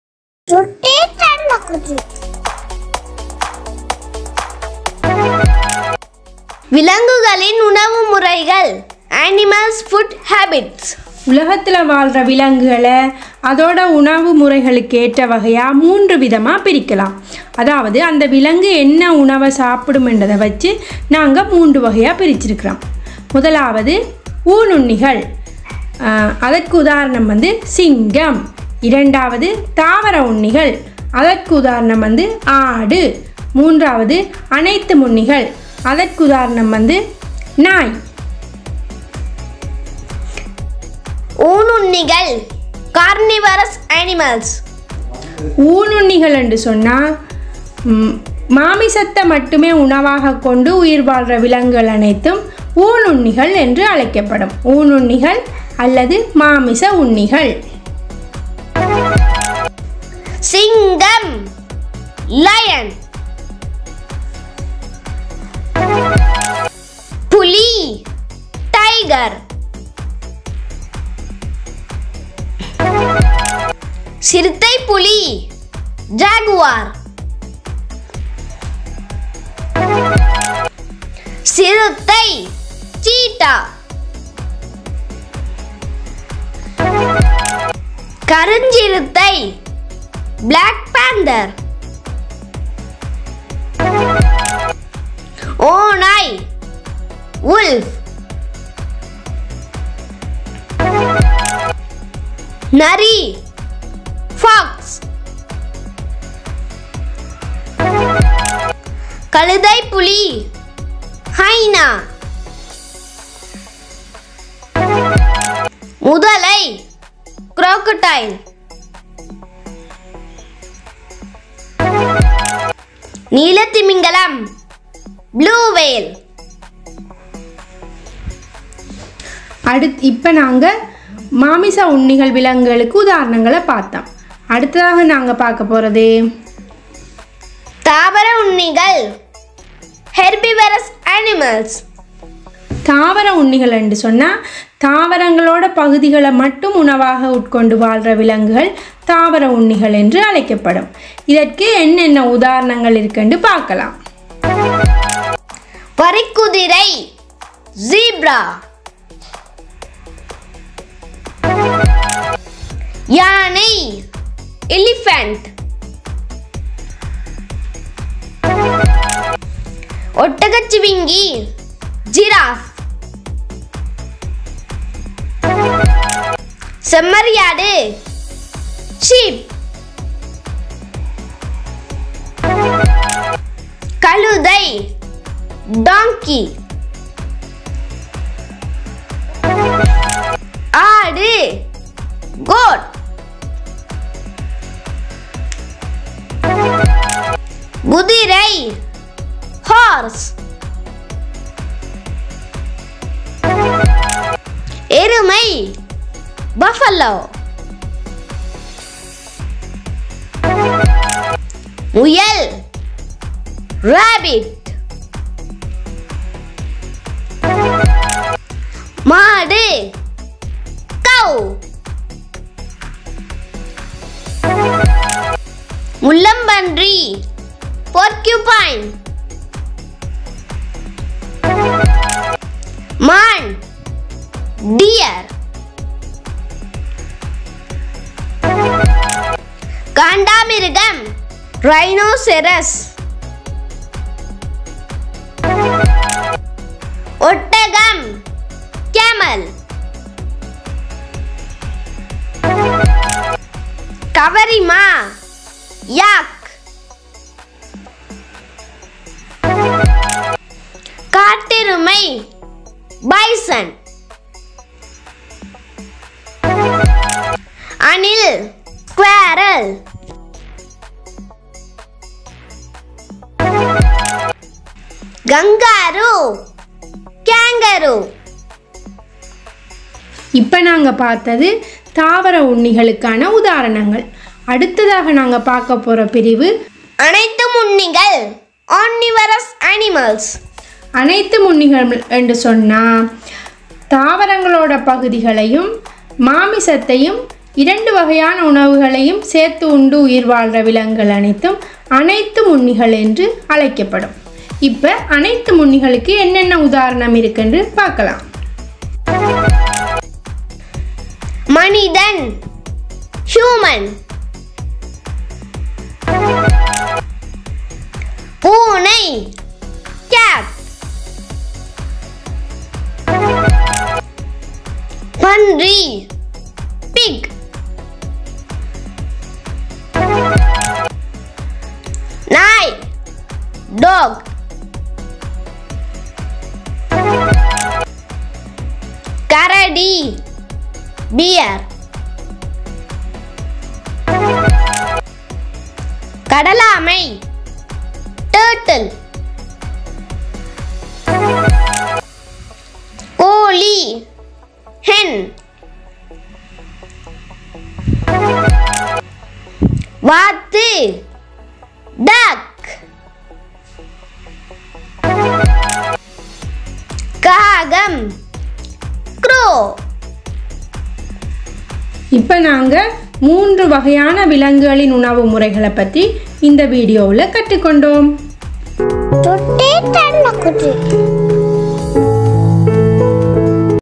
விலங்குகளின் உணவு முறைகள்/Learn Animals food habits with English and Tamil
சுட்டி செல்லக்குட்டி

உலகில் வாழும் விலங்குகள் பல்வேறு வகையான உணவு முறைகளை கொண்டுள்ளன எனவே அவற்றின் வகைகளை குழந்தைகளுக்கு தெளிவாக சொல்வதுடன் விலங்குகளையும் அவற்றின் உணவு முறைகளையும் தனித்தனியே தெரிந்து கொள்ளக் கூடிய வகையிலும் இக்காணொளி உருவாக்கப்பட்டுள்ளது.

This video made for teach children about variety of animal food habits.
Our animal kingdom has 3 types of food habits. This video contains examples pictures for these three types of animal food habits.
From this video children will learn about animal food habits with examples